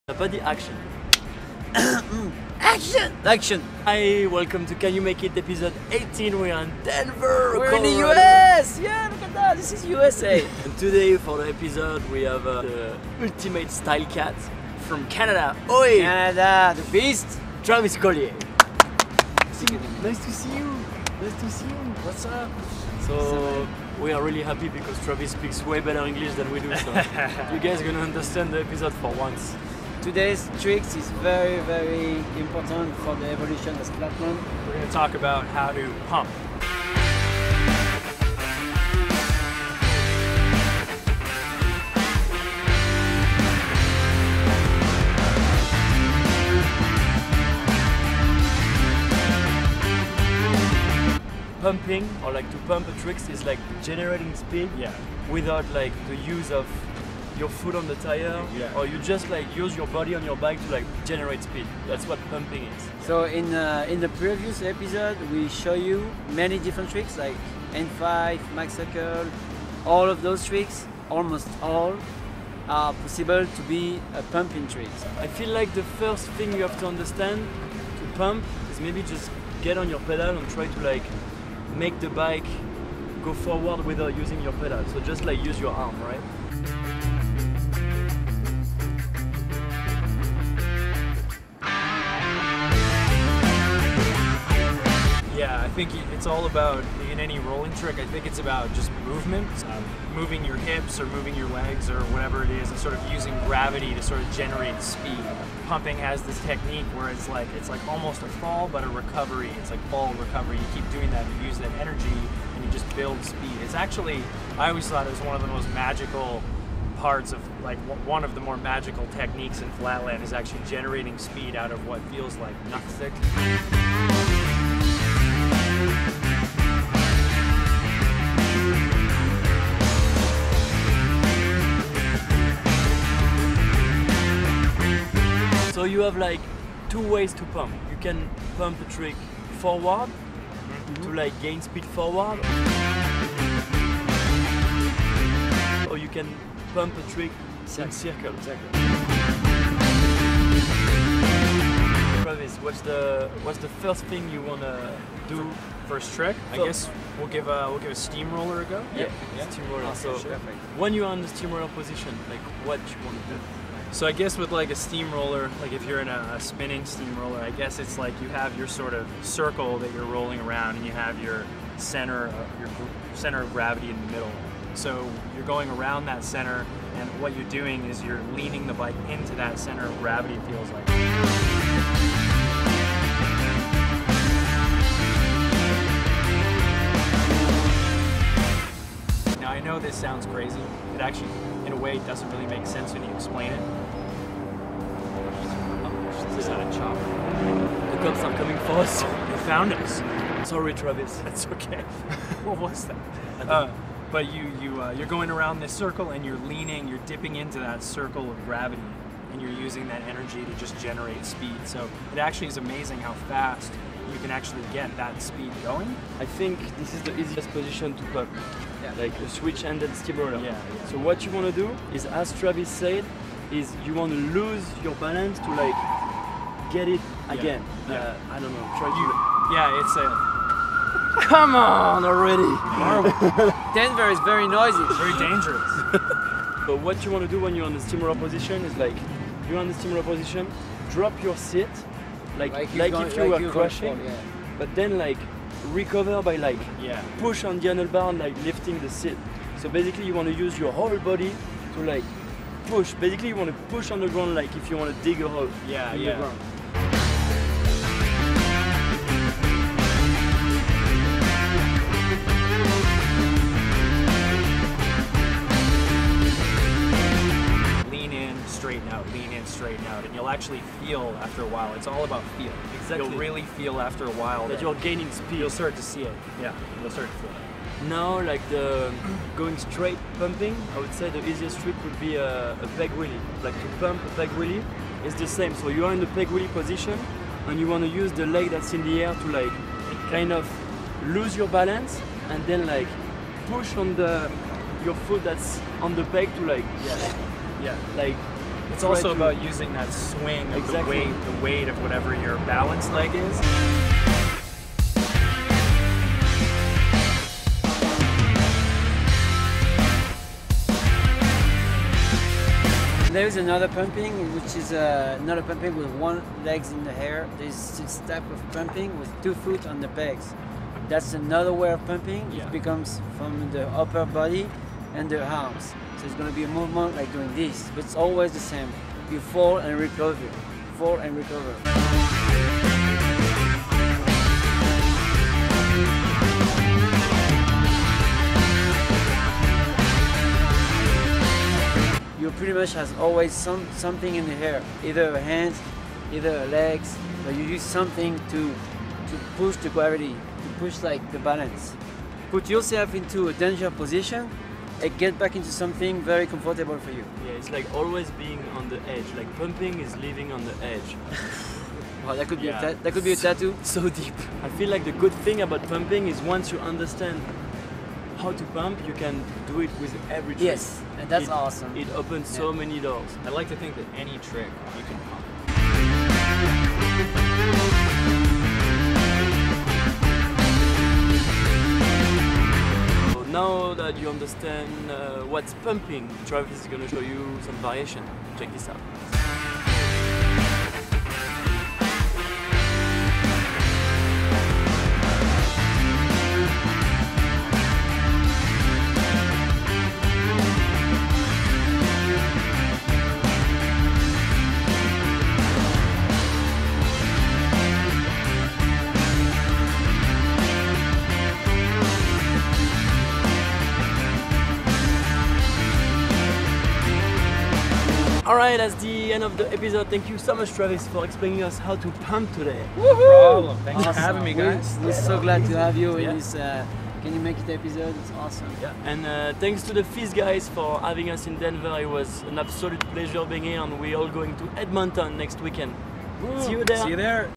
How to "Pumping" | BMX Flatland | Can you make it! episode 18
In this new episode of Can you make it! Matthias Dandois and Alex Jumelin receive the ultimate stylecat from Canada: Travis Collier to teach you how to do one of the most important flatland technique: the pumping!
Watch, learn, and go out there to try!!
Filmed with The Z camera by E1